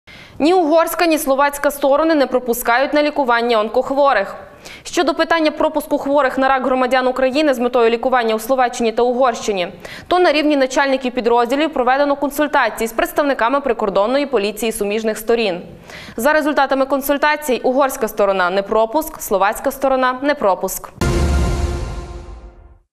Закритий для онкохворих
Ні угорська, ні словацька сторони не пропускають на лікування онкохворих. Щодо питань пропуску хворих на рак громадян Украіни з метою лікування у Словаччині та Угорщині – то на  рівні начальників підрозділів проведено консультаціі з представниками прикордонноі поліціі суміжних Сторін.